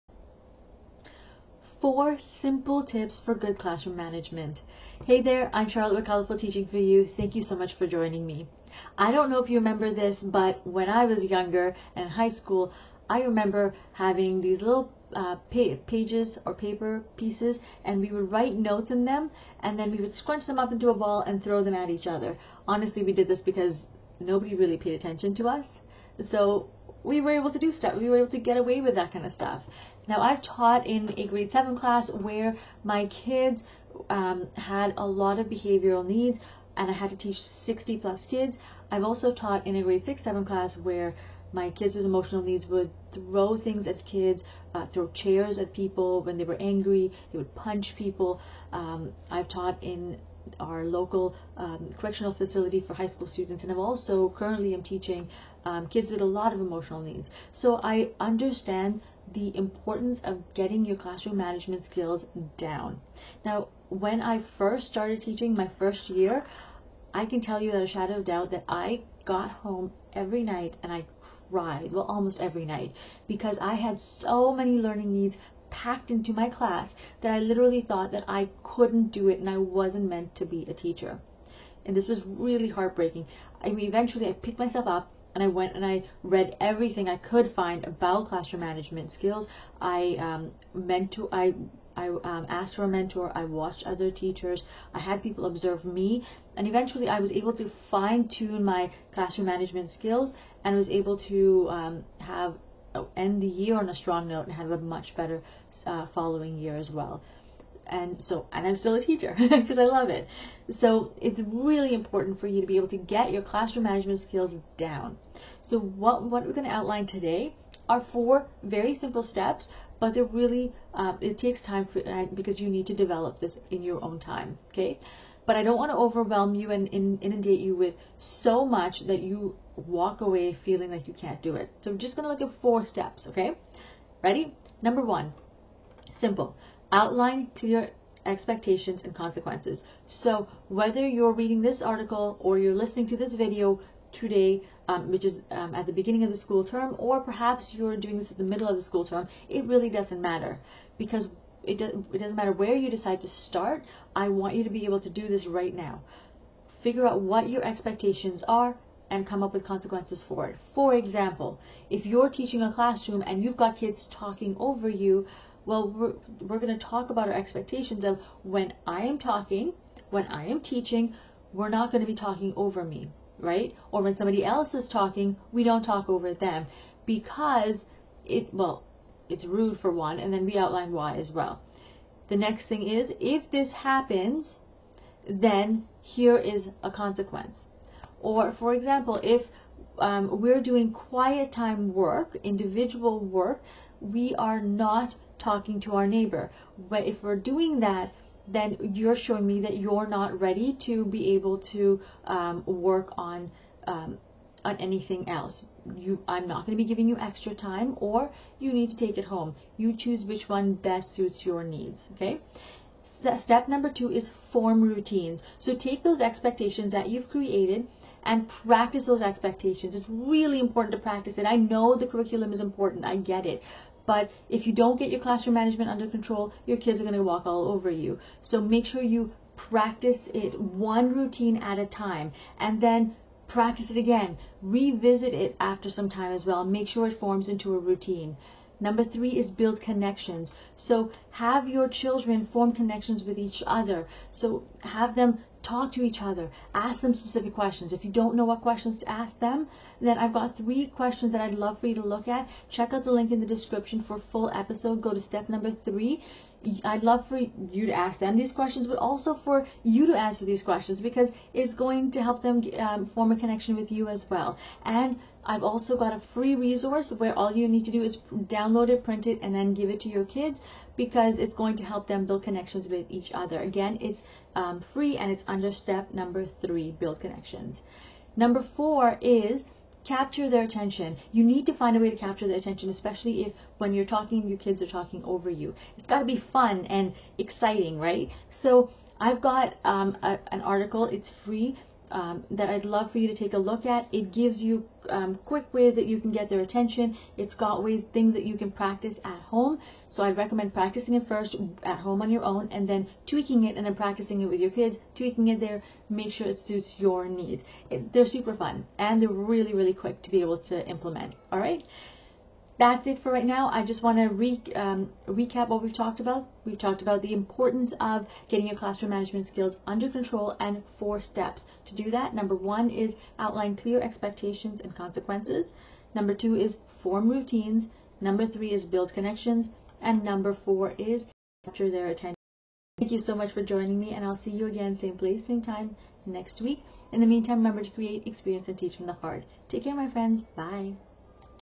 Classroom Management Strategies and Tips for Elementary, Middle and High School
4 Simple Tips for Good Classroom Management.